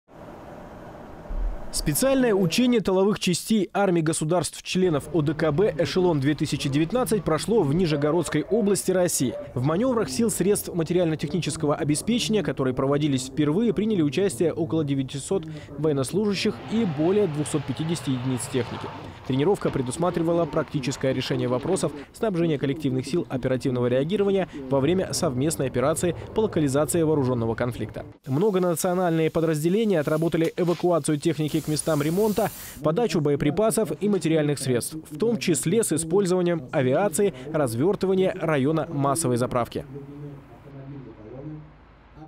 Учение ОДКБ «Эшелон-2019»
Специальное учение тыловых частей армий государств – членов ОДКБ «Эшелон-2019» прошло в Нижегородской области России.

В маневрах сил и средств материально-технического обеспечения, которые проводились впервые, приняли участие около 900 военнослужащих и более 250 единиц техники.